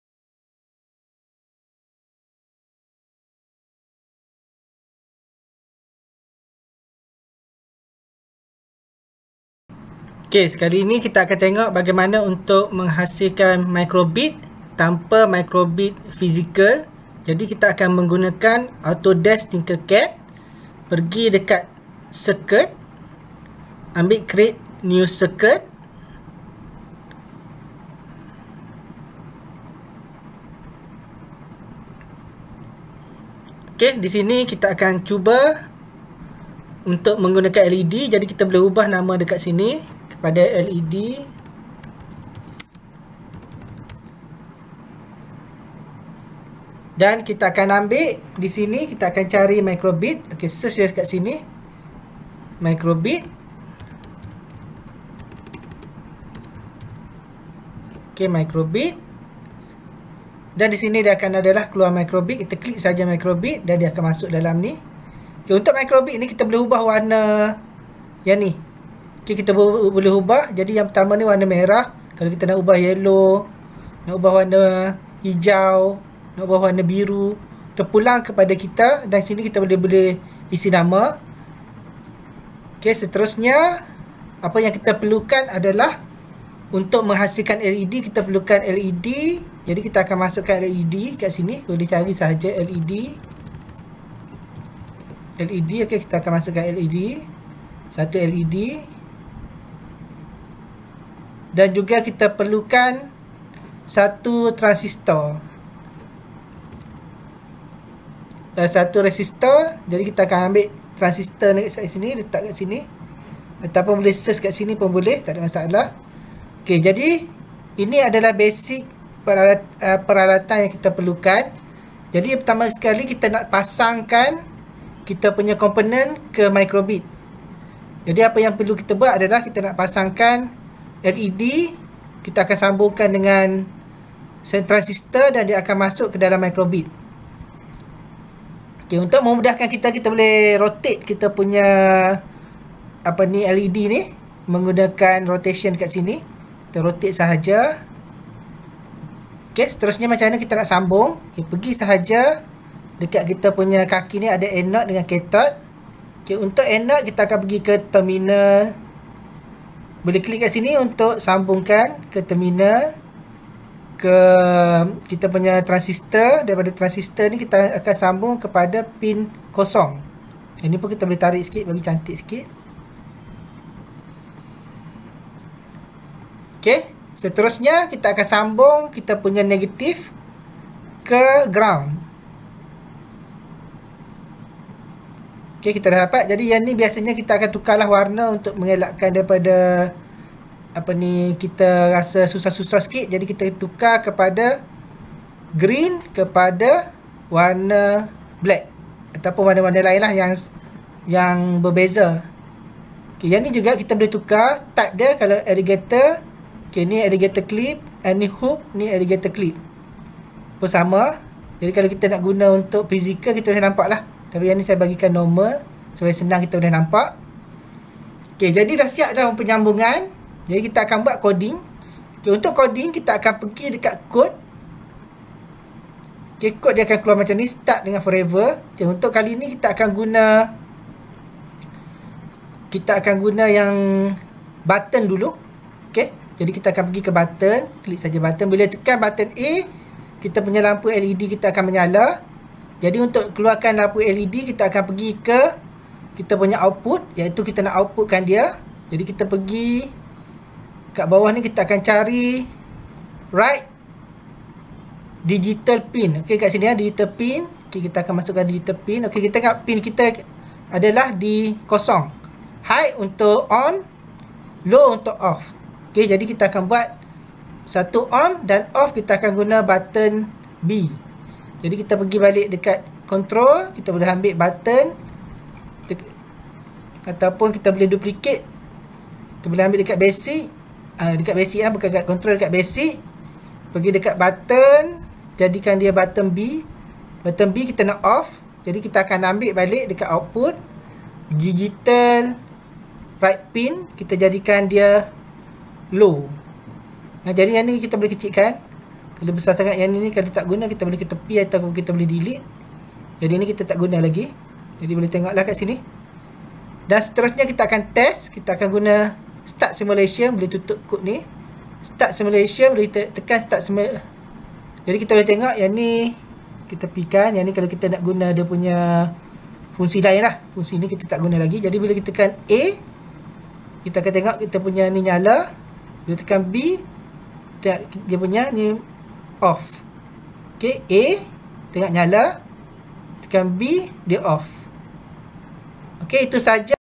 Pengenalan kepada Pengkomputeran Fizikal Siri 1 LED menggunakan Microbit di Aplikasi Thinkercad
Menggunakan Aplikasi Thinkercad untuk membina arahan Micro:bit secara maya.